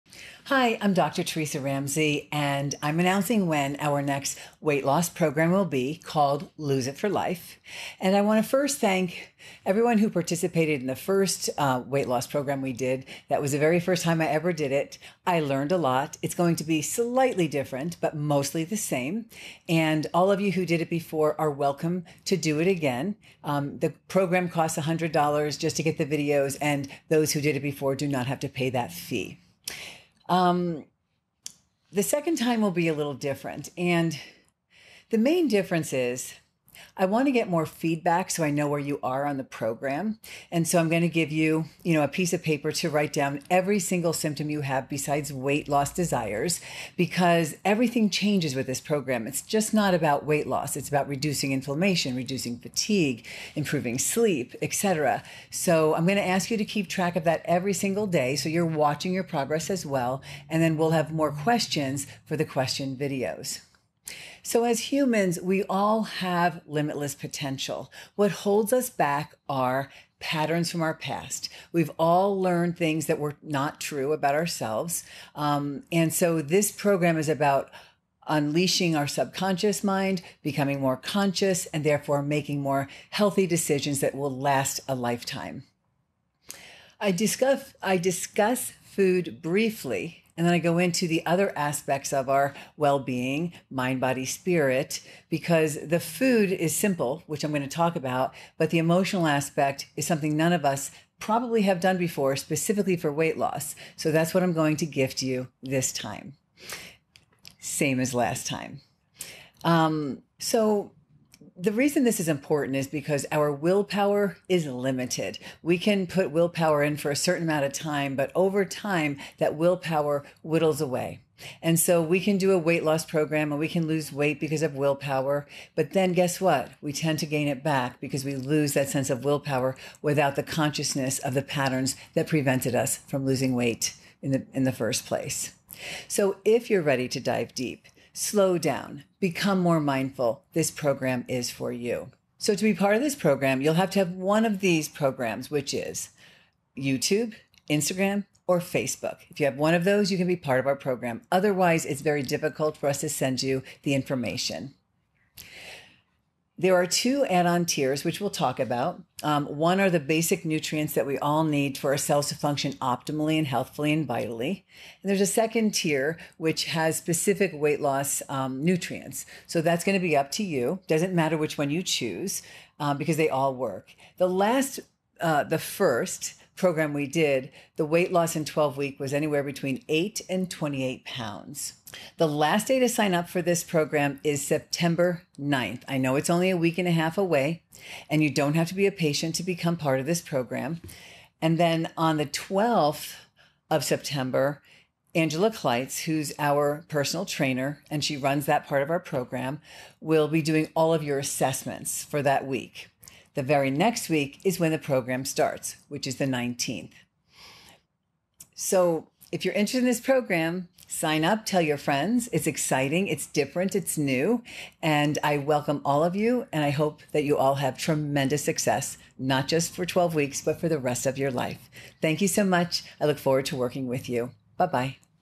"Lose it for Life" Program Round 2
Dr. Ramsey talks about the "Lose it for Life" weight loss program that will be running once again this fall. She goes over the benefits, some of the results from the first program, and what to expect from the initiative.

The sign up deadline is Friday, September 9th. Angela Kleitz will then begin reaching out to the participants the following Monday the 12th for the pre-program assessment. The program will then officially begin Monday September 19th.